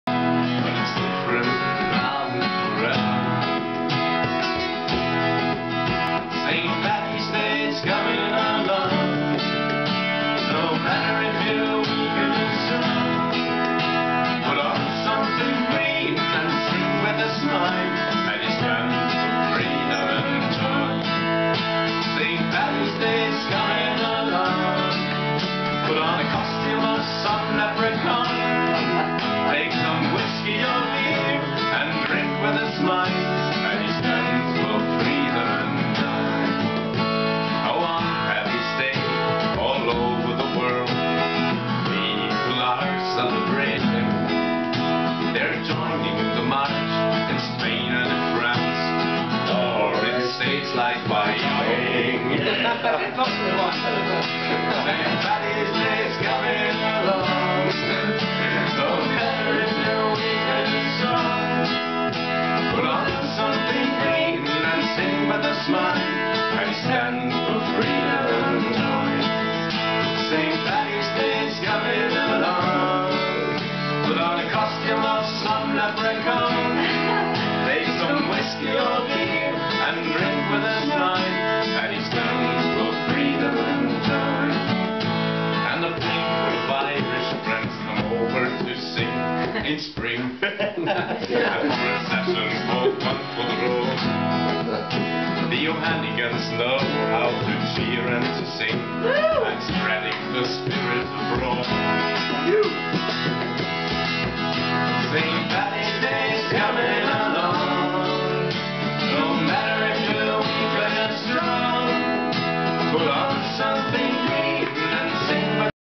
kats photos 029
christian and norbert,singing a song they wrote about paddy's day,